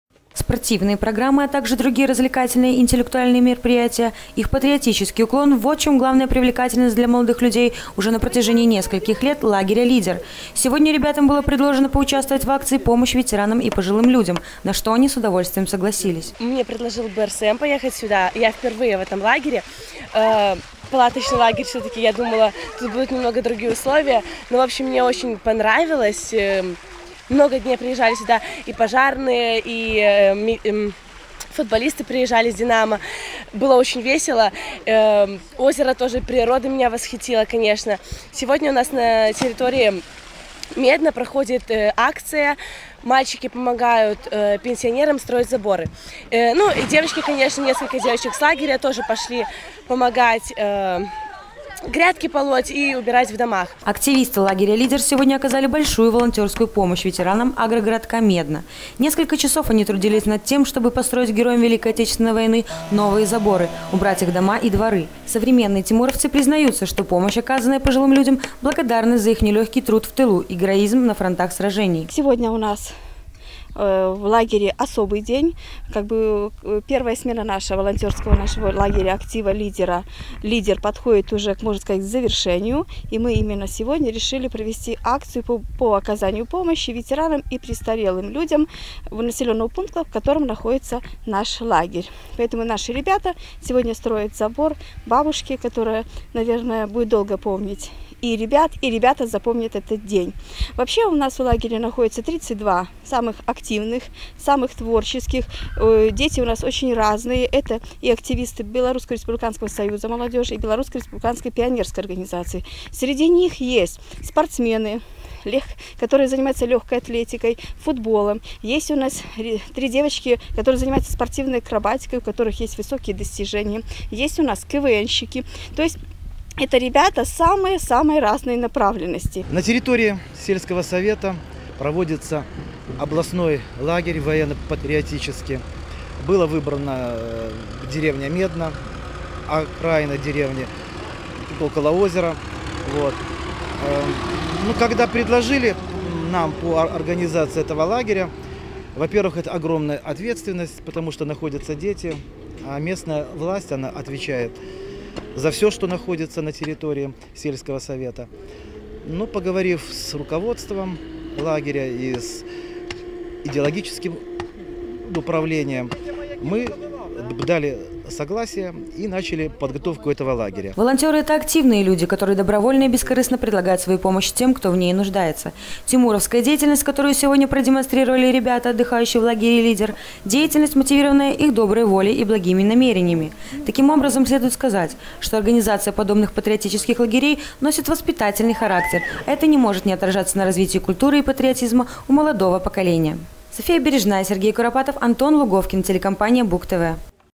2014-07-15 г. Брест Телекомпания Буг-ТВ. Трудовые будни лагеря «Лидер -- Патриот» в д. Медно
Журналист С. Бережная. Оператор С. Куропатов
Трудовые будни  оздоровительного молодежного  лагеря «Лидер -- Патриот» в д. Медно Брестского района.
Кто-то в  мечтах о будущем студенчестве, другие  -  активно отдыхают, наслаждаясь песнями под гитару, обедом с котелков и массой других приятных неожданностей.Молодежный лагерь «Лидер-Патриот», который в этом  году расположился на лесной поляне у озера Медное, начал свою новую смену 8 июля. Лагерь актива существует благодаря Брестской областной организации  «БРСМ» при поддержке облисполкома уже на протяжении 17 лет. Каждый год в уютно обустроенный десантниками палаточный городок приезжают  самые инициативные, спортивные и творческие ребята.2014-й летний сезон не стал исключением. Сегодня лагерь встречает настоящих лидеров, юношей и девушек, для которых активный отдых --лучший способ проведения каникул.